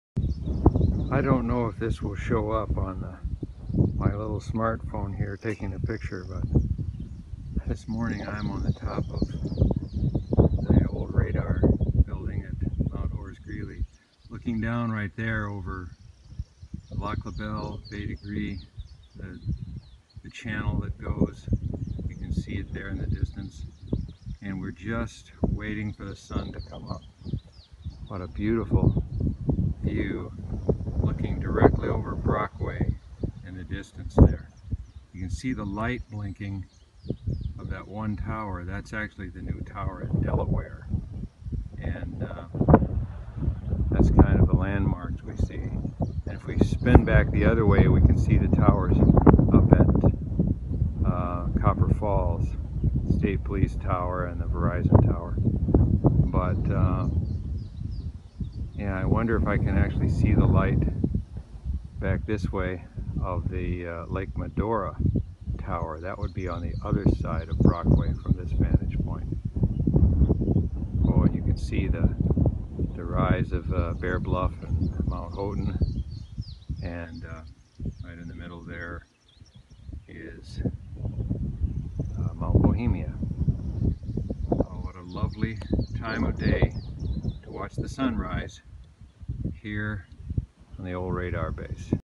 Mount Horace Greeley Sunrise
Viewed from the top of the old radar building, rising over Brockway.